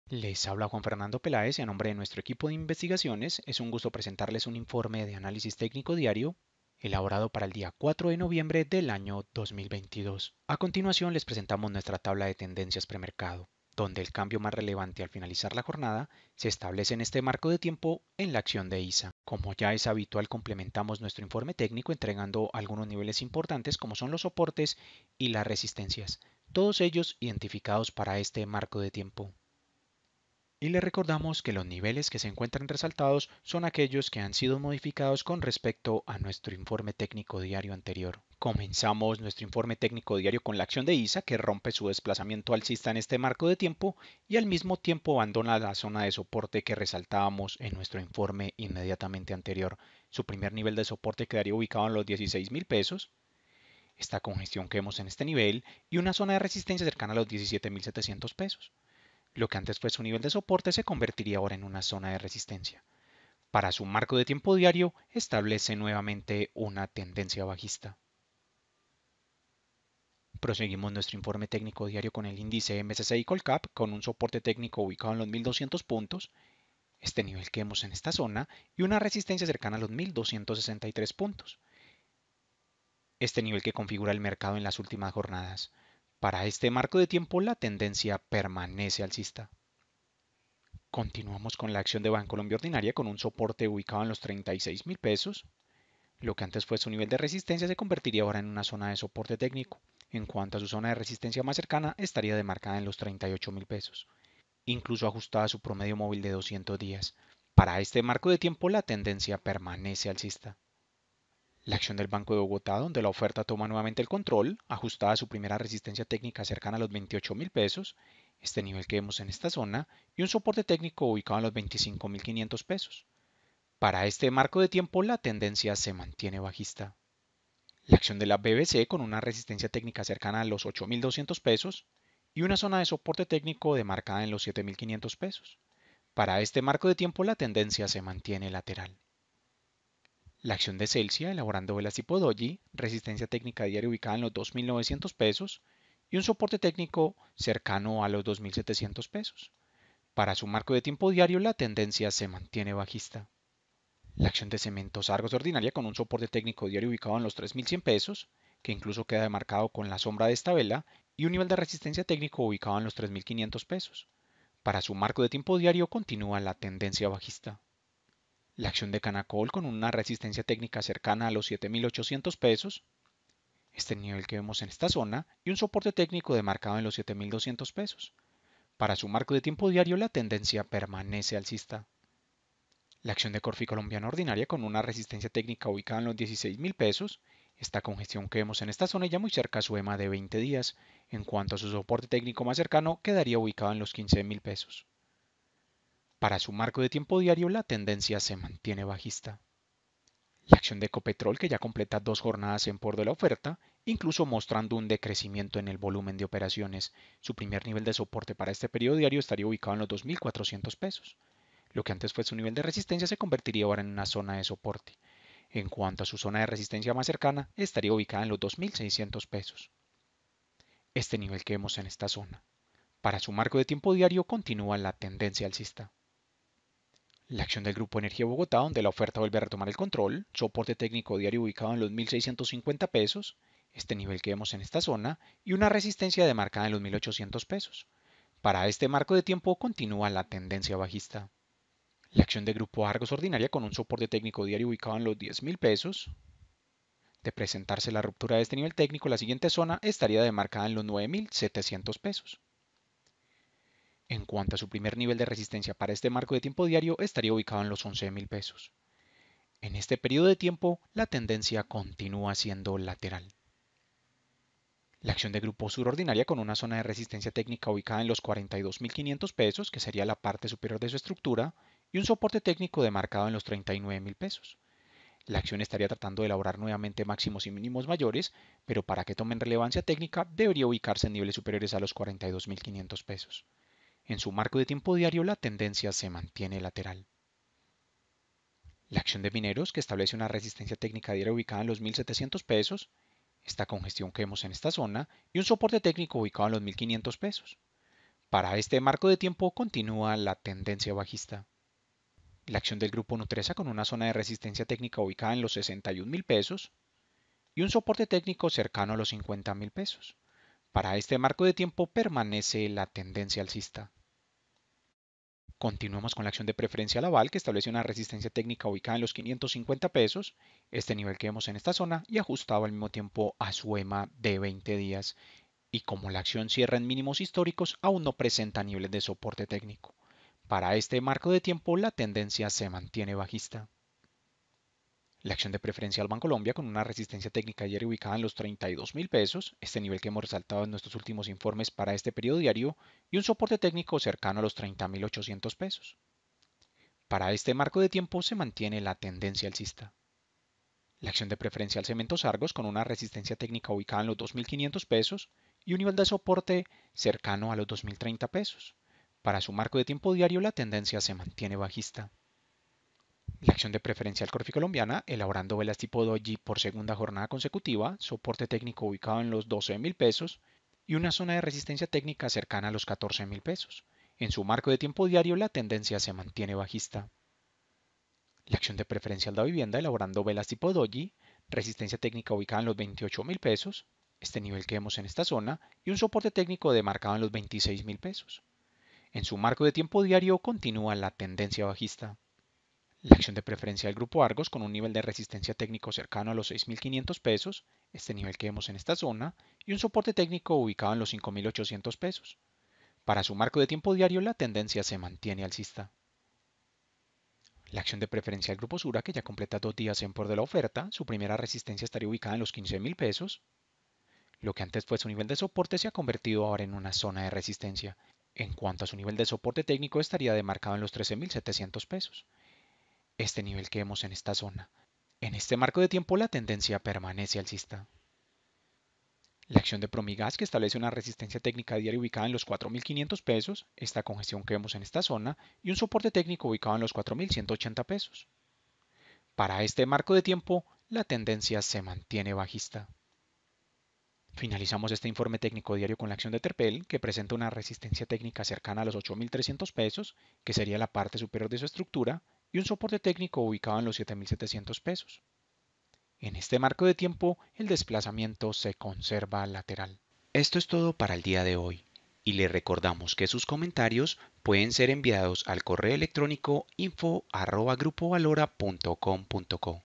ISA y PF Grupo Sura las acciones que más caen en la jornada noviembre 04/22
El índice msci colcap de la Bolsa de Valores de Colombia finaliza la jornada en 1.215,51 puntos marcando una variación negativa de – 2.38% y completa dos días de retroceso consecutivos. El acumulado anual del índice se conserva negativo y marca una variación (YDT) de – 13.85 %.
Desde nuestra perspectiva técnica el índice msci colcap comienza una tendencia alcista en el marco de tiempo diario.  La zona de soporte quedaría establecida en los 1.200 puntos. En cuanto a su primera resistencia se ubica en los 1.300 puntos, una congestión que establece el mercado en las últimas jornadas.
Al finalizar la rueda de negocios el mercado de acciones colombiano registró un volumen diario de operaciones de $ 36.669 millones, mostrando una importante variación negativa de - 49.9 % con respecto a la jornada inmediatamente anterior cuando se movilizaron $ 73.211 millones. Las últimas dos jornadas de retroceso se ha marcado una importante contracción en el volumen de operaciones.
Los tres títulos más activos en temas de volumen durante la rueda de negocios fueron: La petrolera estatal ECOPETROL (ECOPETROL - 1.57%) se ubica en primer lugar en este ítem movilizando $9.915 millones al finalizar el día. En el segundo puesto encontramos las acciones de Bancolombia preferencial (PFBCOLOM - 3.11%) que negocio $8.593 millones. Finalmente, en la tercera casilla la empresa de interconexión eléctrica Isa (ISA - 7.49%) que transo un monto de $4.124 millones.
En una jornada que se marcó una corrección para el índice msci colcap, los títulos de la empresa de interconexión eléctrica (ISA) cayeron -7.49% y se ubicaron como el activo con mayor retroceso al cierre de la rueda de negocios.  Las acciones de Grupo sura preferencial (PFGRUPSURA) tampoco tuvieron una buena jornada bursátil, ya que sus títulos presentaron una bajada de -4.22%. Finalmente, tenemos las acciones de Bancolombia ordinaria (BCOLOMBIA) que   cayeron -3.72%.